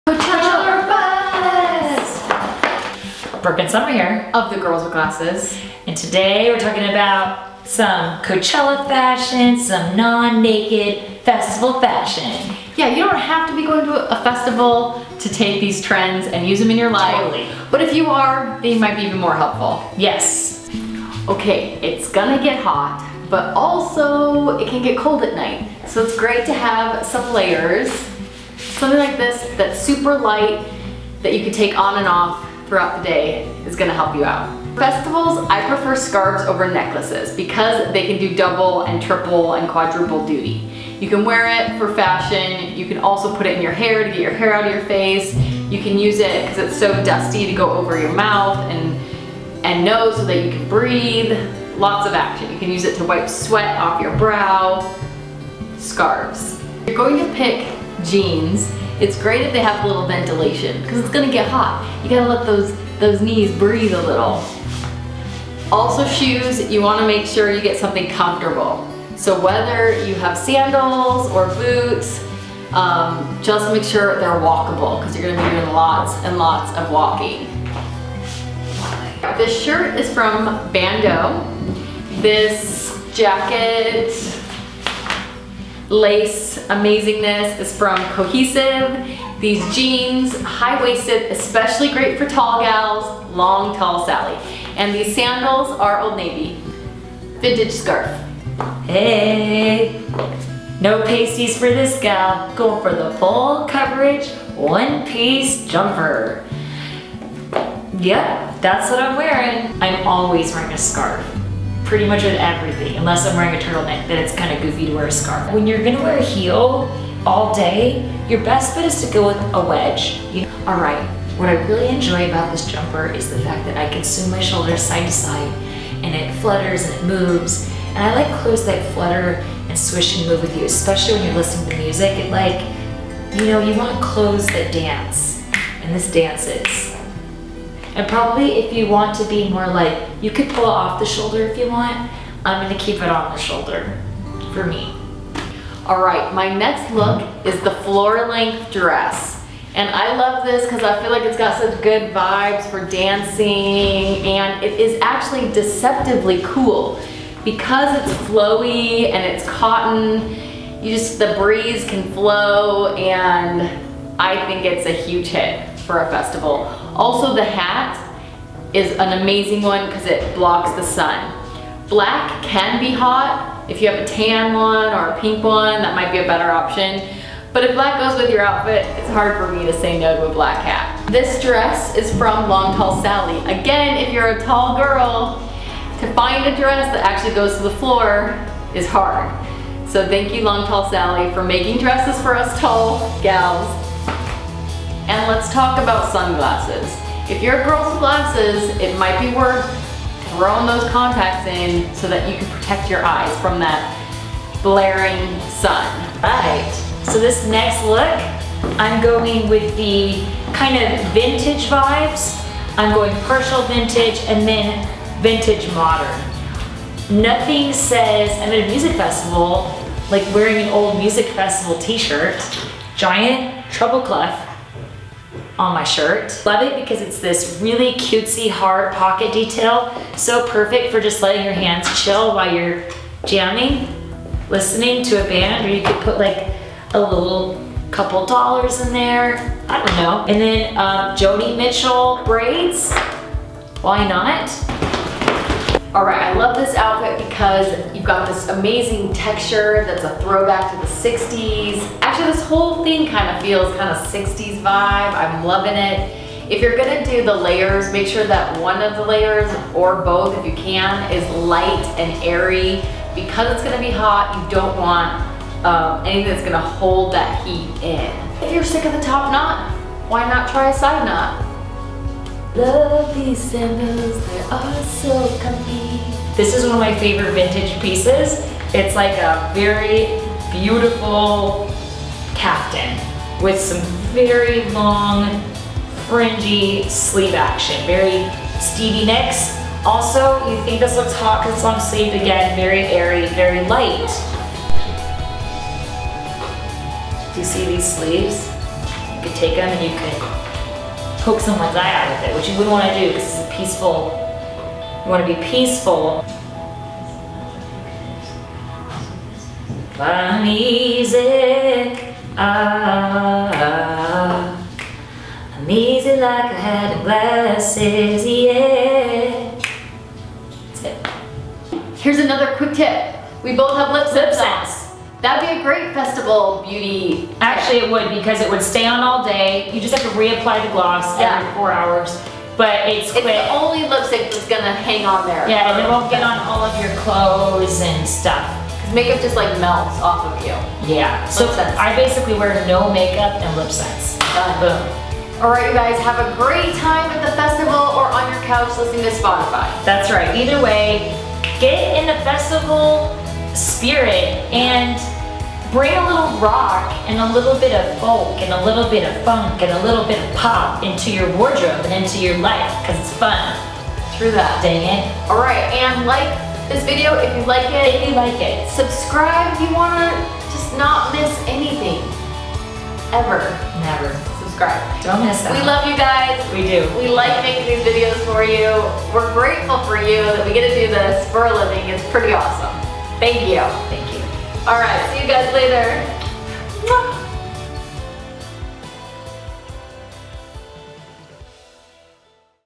Coachella Fashion - What to Wear to a Music Festival
Here are some of our looks:
Summer 1st:

Brooke 2nd:
Tee - vintage

Dress - vintage